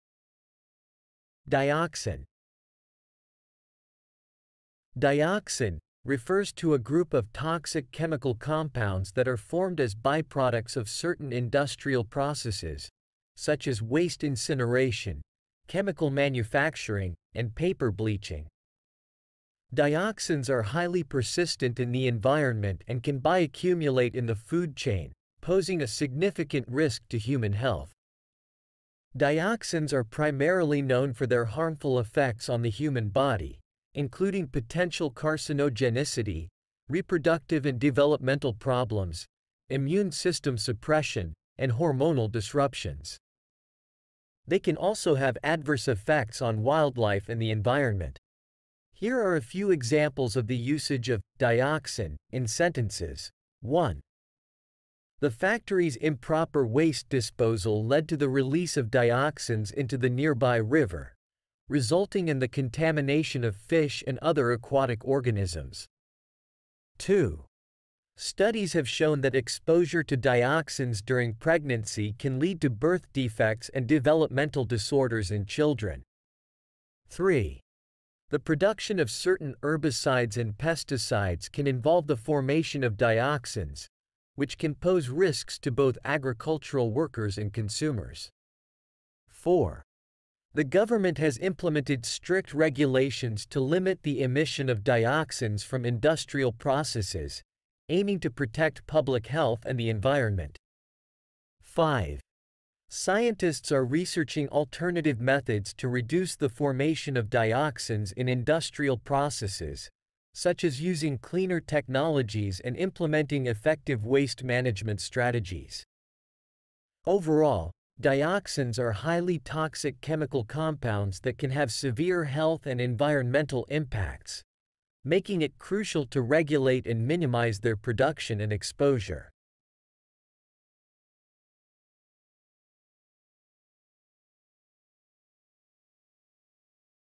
Dioxin
"Dioxin" refers to a group of toxic chemical compounds that are formed as byproducts of certain industrial processes, such as waste incineration, chemical manufacturing, and paper bleaching. Dioxins are highly persistent in the environment and can bioaccumulate in the food chain, posing a significant risk to human health.
Dioxins are primarily known for their harmful effects on the human body, including potential carcinogenicity, reproductive and developmental problems, immune system suppression, and hormonal disruptions. They can also have adverse effects on wildlife and the environment.
Here are a few examples of the usage of "dioxin" in sentences:
1. The factory's improper waste disposal led to the release of dioxins into the nearby river, resulting in the contamination of fish and other aquatic organisms.
2. Studies have shown that exposure to dioxins during pregnancy can lead to birth defects and developmental disorders in children.
3. The production of certain herbicides and pesticides can involve the formation of dioxins, which can pose risks to both agricultural workers and consumers.
4. The government has implemented strict regulations to limit the emission of dioxins from industrial processes, aiming to protect public health and the environment.
5. Scientists are researching alternative methods to reduce the formation of dioxins in industrial processes, such as using cleaner technologies and implementing effective waste management strategies.
Overall, dioxins are highly toxic chemical compounds that can have severe health and environmental impacts, making it crucial to regulate and minimize their production and exposure.